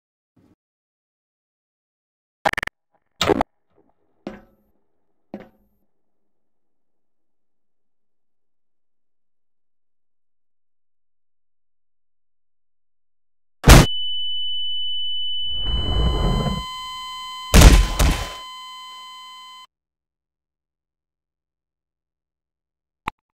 Ass Rape-inator Fail
Max attempts to build his Ass Rape-inator rocket launcher, when it fails predictably. I have no sympathy for him.